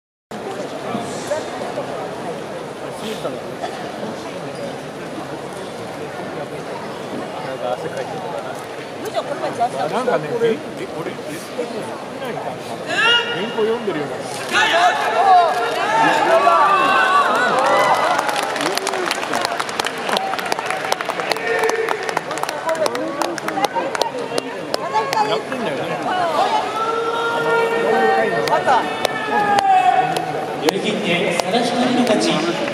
Okinoiwa (4-3) - Sadahikari (5-2) , Sumo : Natsubasho '16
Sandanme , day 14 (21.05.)
(l) Okinoiwa - Sd 50w, Hakkakubeya
(r) Sadahikari - Sd 46w, Sakaigawabeya